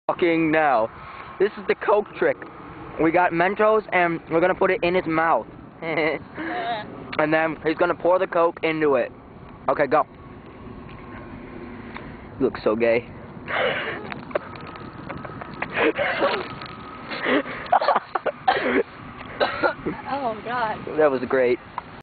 I try Mentos and Diet Coke...in the MOUTH!
I..um...try mentos and diet coke in the mouth